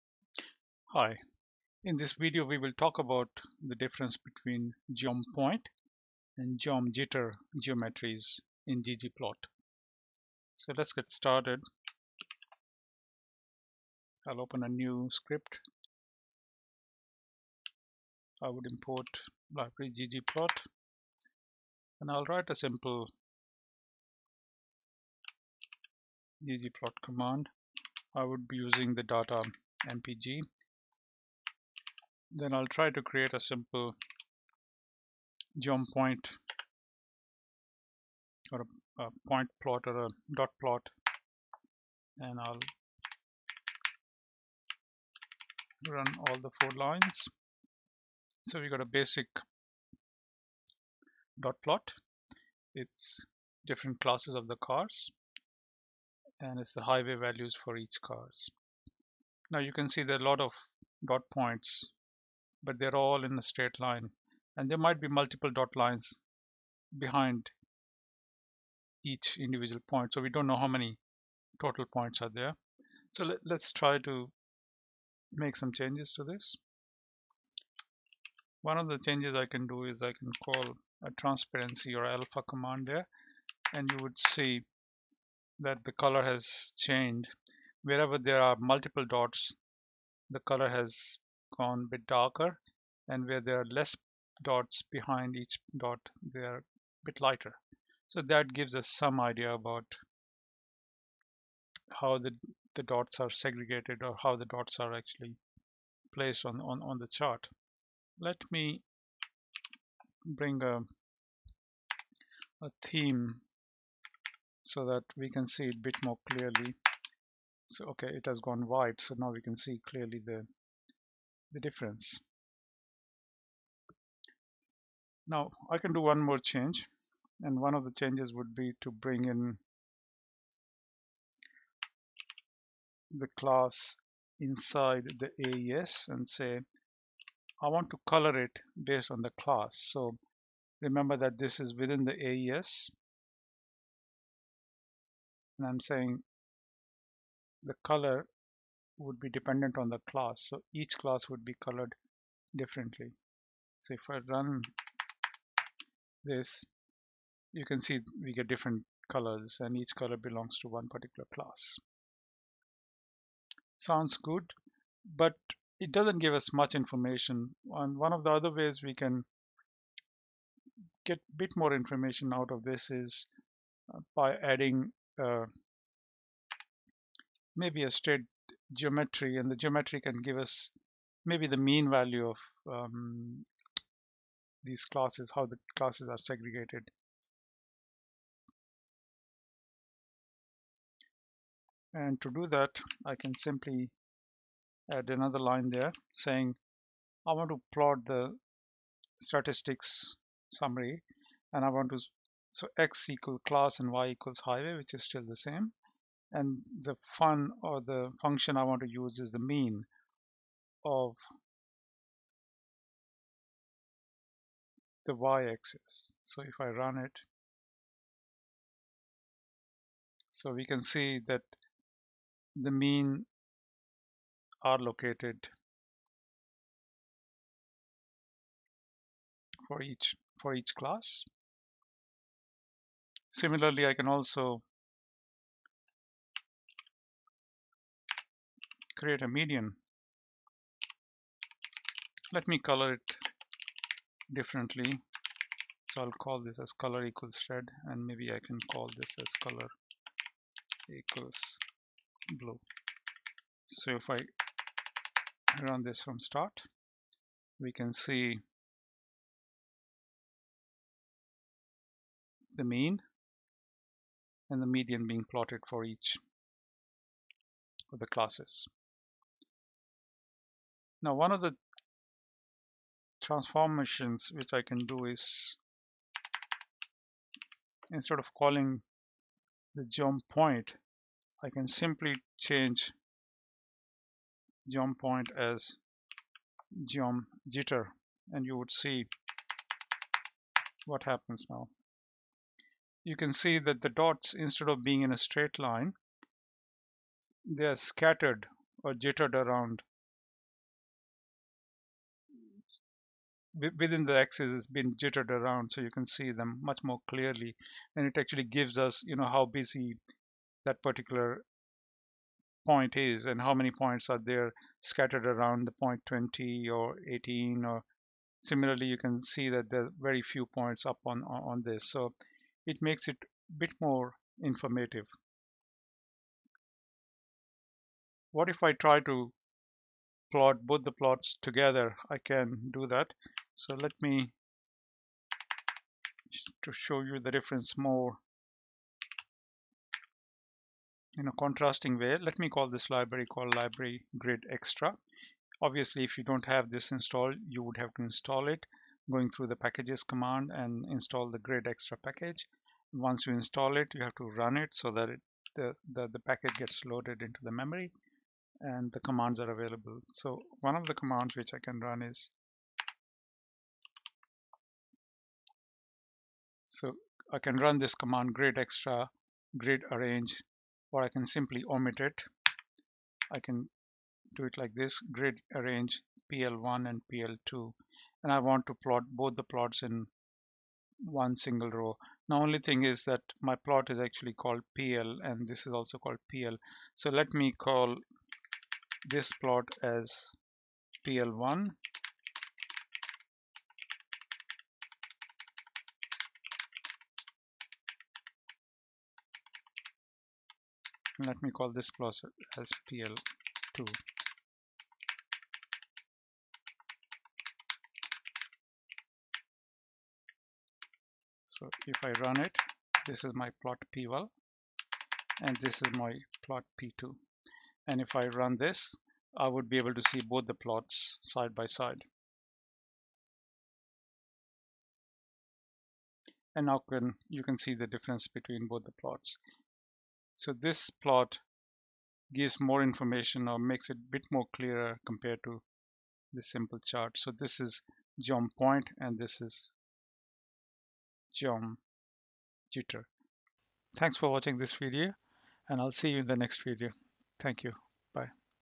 Rstudio:Beginners - what is geom_point and geom_jitter in ggplot in R.
Basic guide to plot a dot plot in R.Learn to plot Geom_Point and Geom_Jitter/Quickly see what is the difference between Geom_Point and Geom_Scatter.

00:00 Geom_point and Geom_jitter
00:30 Geom_point
4:20 Geom_jitter
5:35 use grid to plot charts side by side